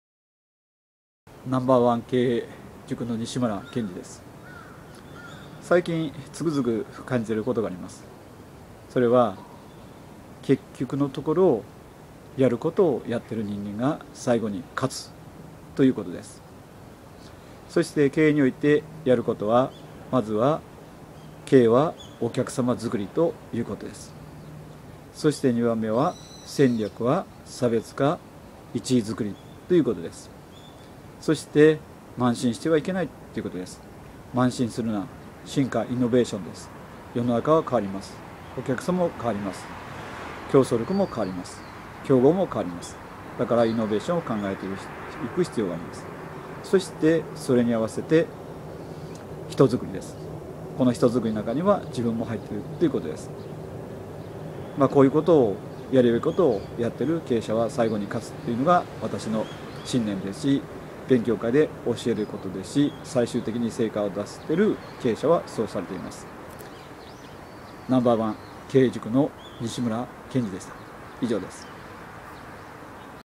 結局のところ、やることをやっている人間が最後に勝つ！ランチェスター
今の時代、売上確保や集客には戦略が必要です！東京戦略社長大学・西村謙二（No.1経営塾）